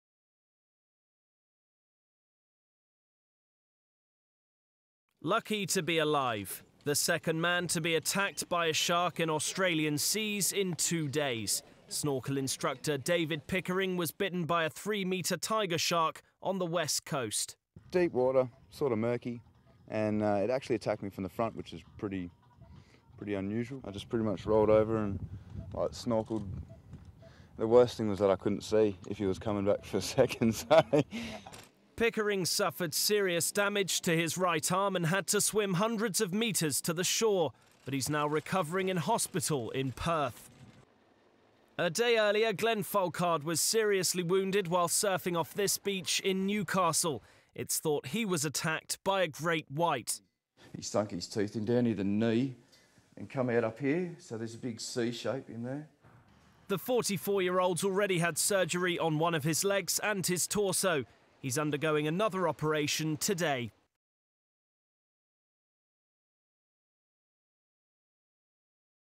Tiger shark mauls snorkel instructor in western Australia
David Pickering was seriously wound by a tiger shark in western Australia, just a day after Glen Folkard was attacked by a suspected great white. Report by Mark Morris.